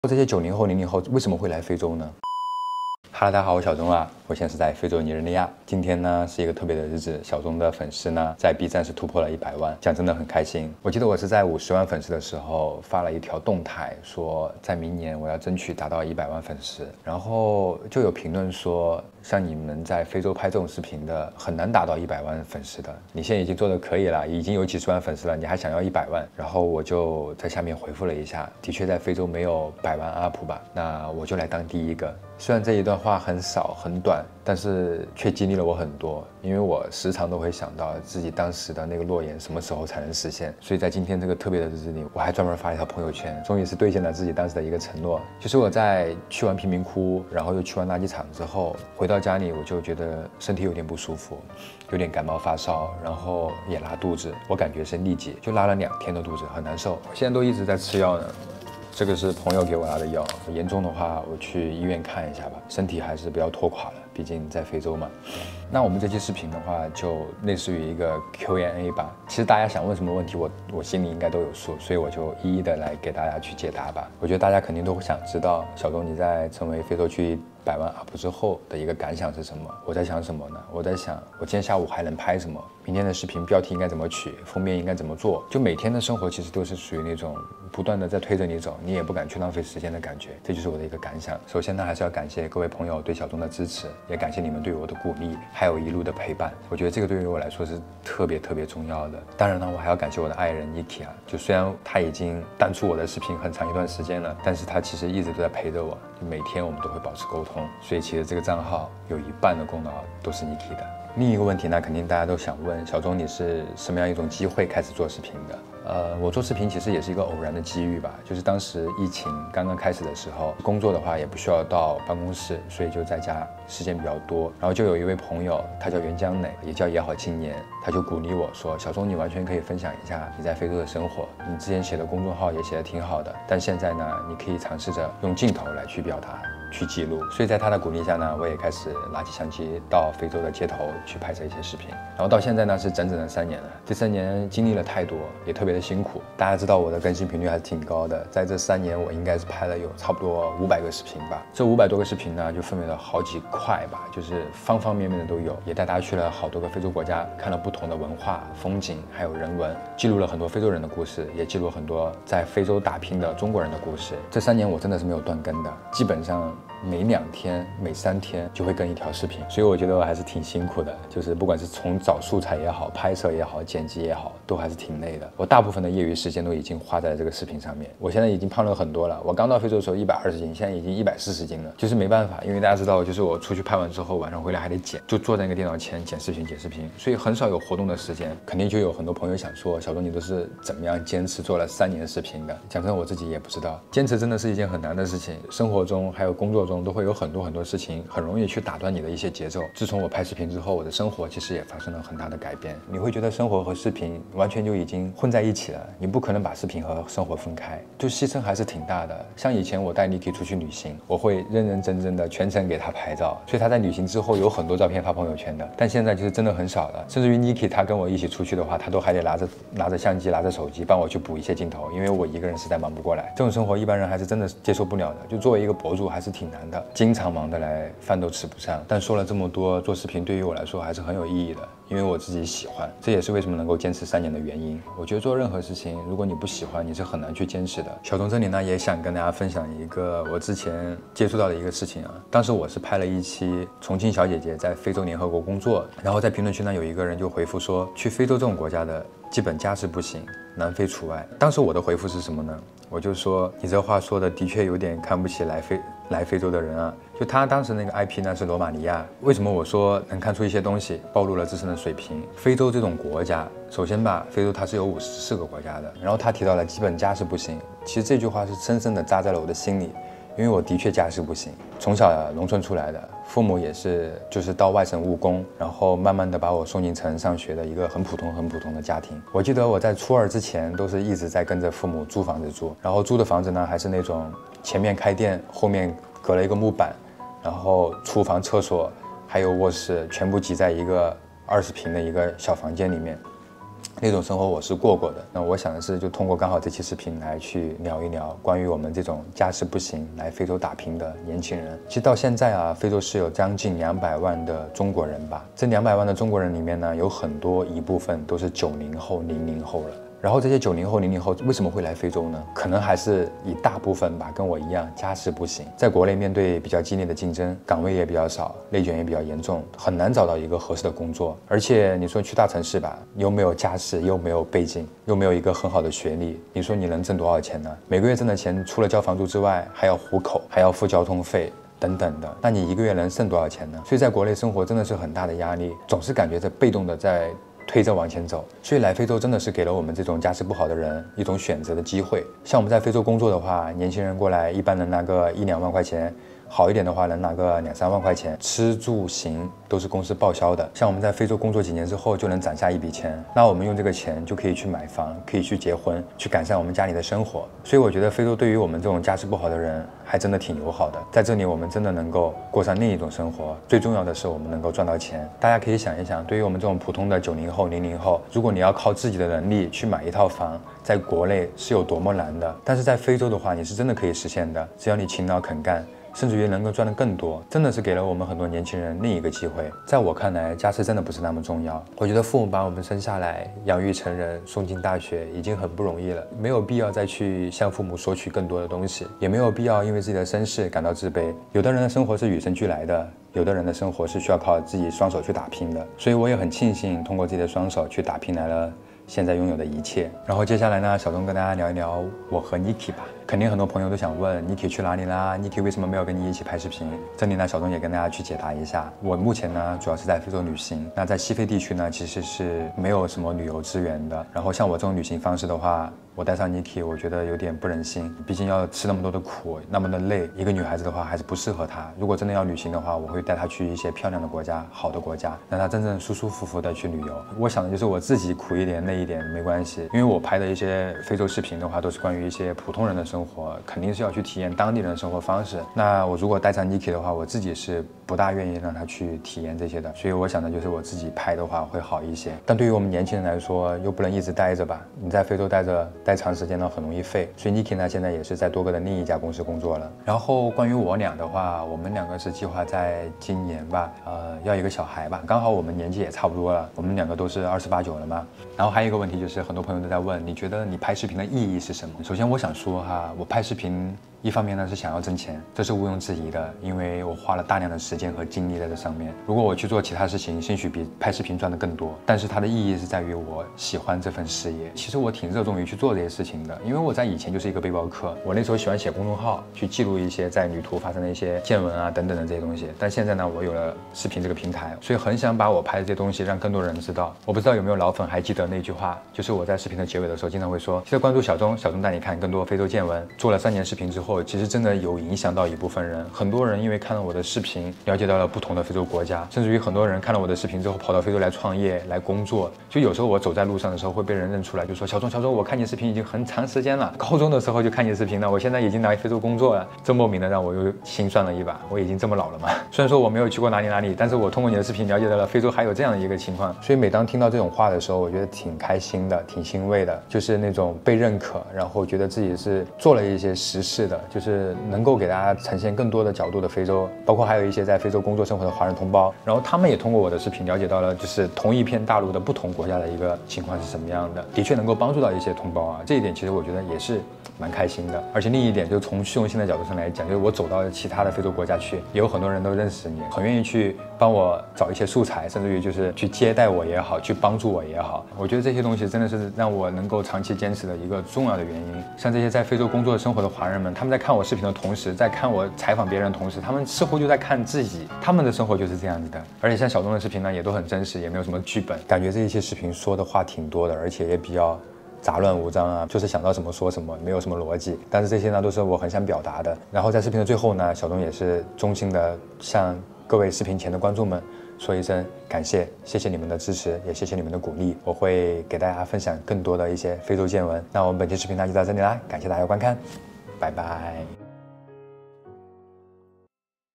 非洲拍视频三年，去了19个国家，终于到100万粉丝，说说心里话
在非洲拍视频三年，去了19个非洲国家，拍了500期视频，在今天终于兑现了自己的承诺！成为了非洲的百万UP主，这三年经历了太多，也成长了很多。今天借这个机会和大家说说心里话。YouTube也要加油了💪早日突破50万。喜欢的小钟视频的朋友们，麻烦你们动动手指给小钟点赞，感谢你们的支持，你的每一个👍都是对我最大支持！❤️非洲旅行